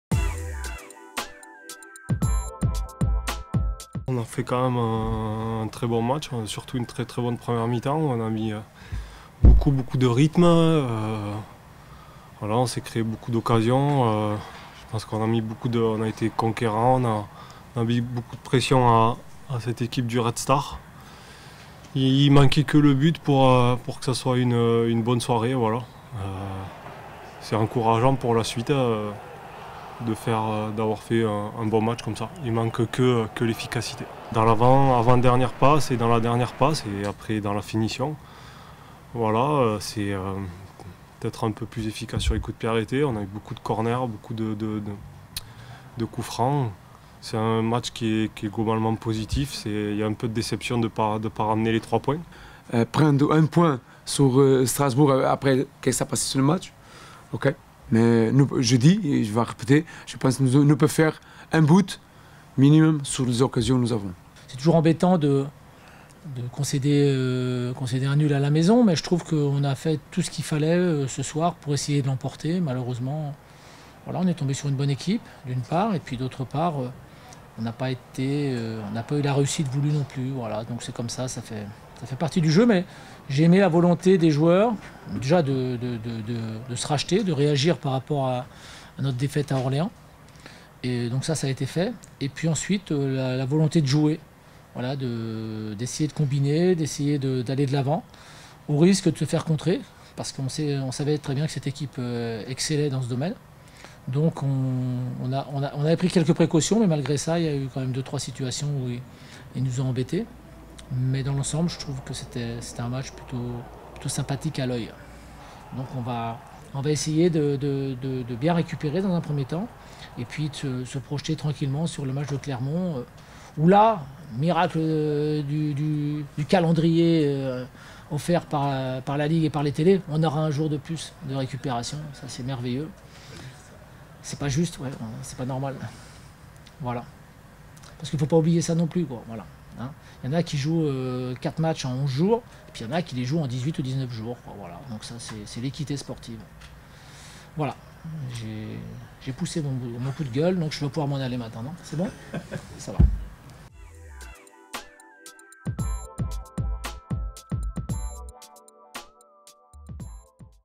RCSA - Red Star (0-0) : les réactions | RC Strasbourg Alsace
Réactions d'après-match de Jérémy Blayac, Thierry Laurey et Rui Almeida, à l'issue de la rencontre du RC Strasbourg Alsace face au Red Star, 8e journée de Domino's Ligue 2.